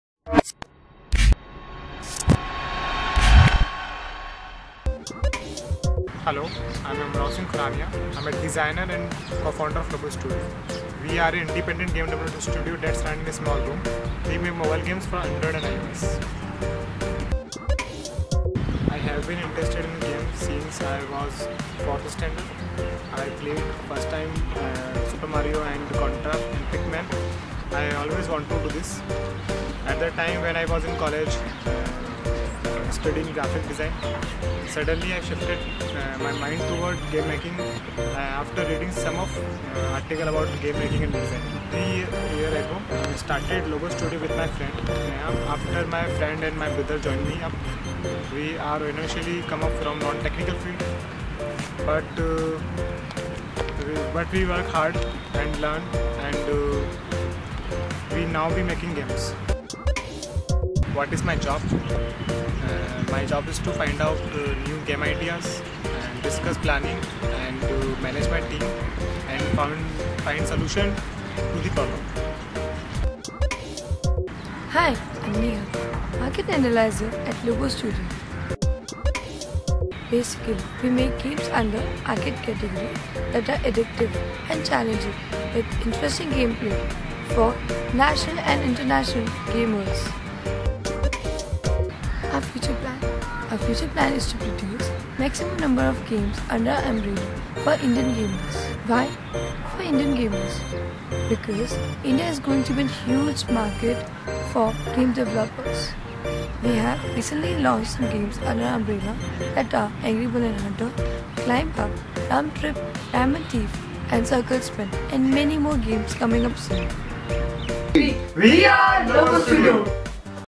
Lobo Studio Story by it's Founder
This vedio is all about the little story of lobo studio that started in a small room with a team of 4 in 2013.